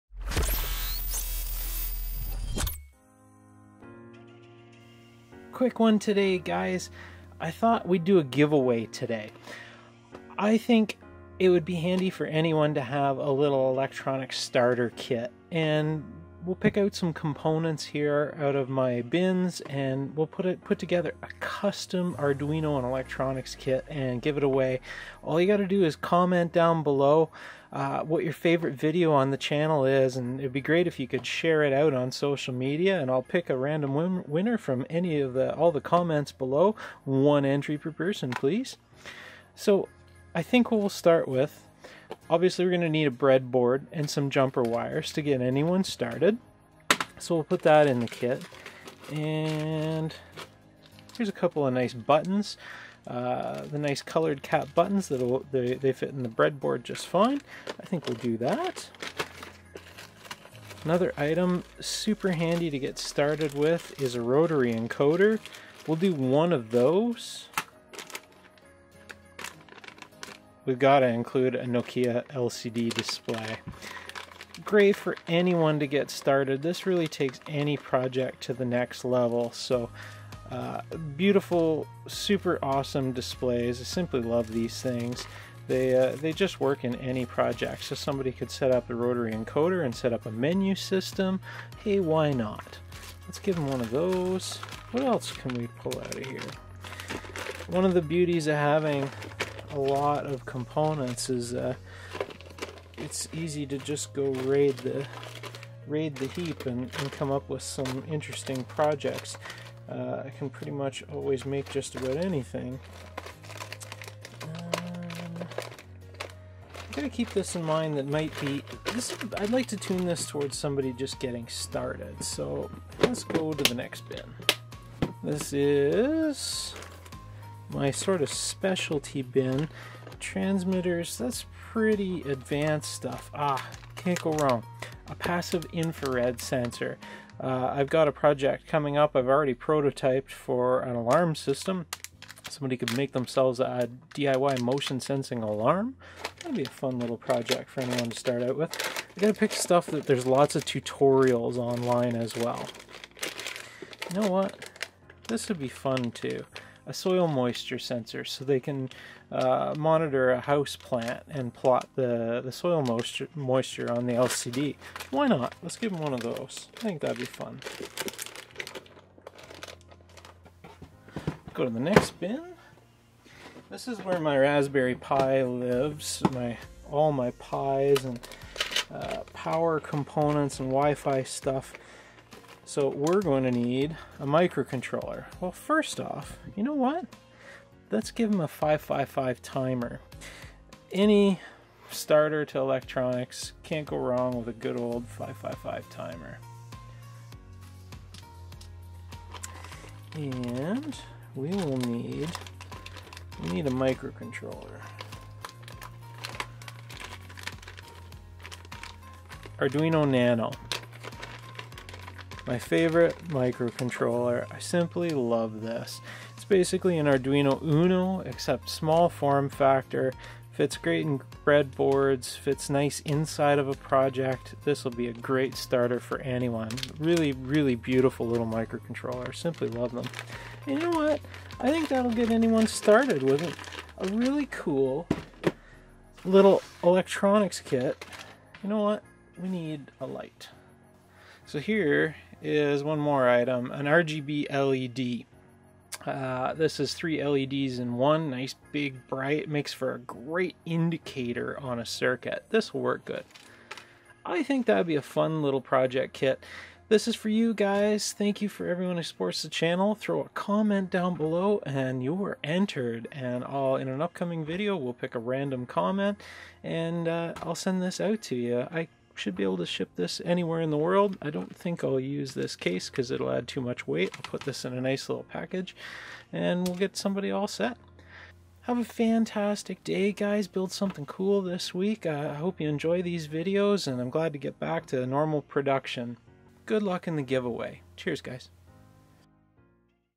FREE Arduino & Electronics Kit! Channel ✔GIVEAWAY✔
In an upcoming video I'll randomly select a connet from this video to win this kit. It should be handy for those new to Arduino/electronics or even advanced hobbyists. The parts are some that I find interesting in my projects and there are many tutorials to be found online for all components.